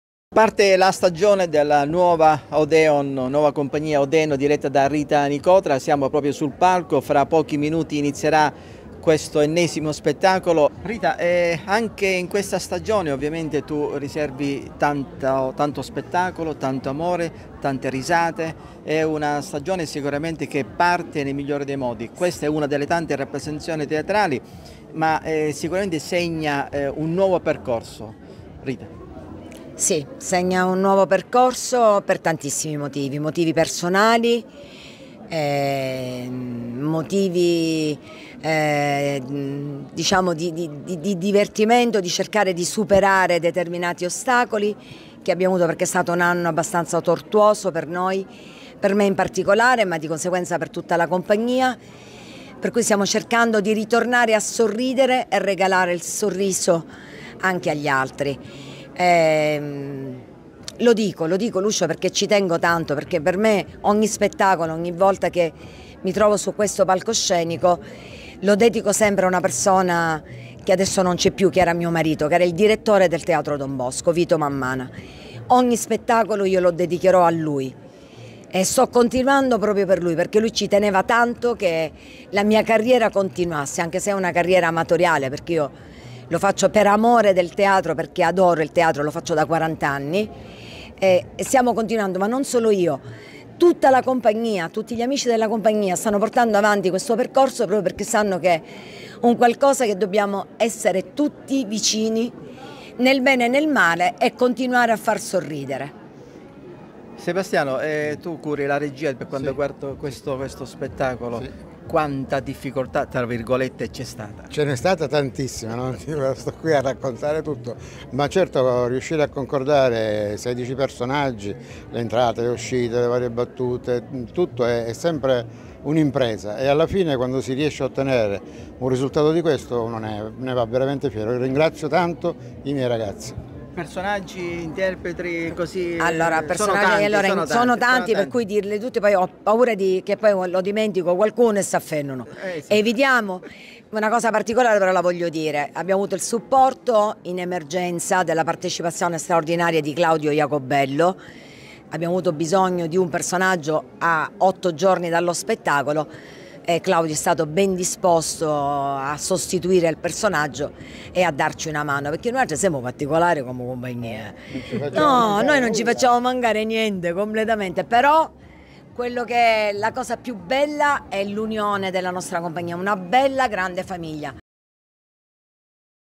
Catania: la Nuova Compagnia Odèon ha messo in scena "Il Morto sta bene in salute"
Al Teatro Don Bosco di Catania, la Nuova Compagnia Odèon ha messo in scena "Il Morto sta bene in salute", regia di Sebastiano Finocchiaro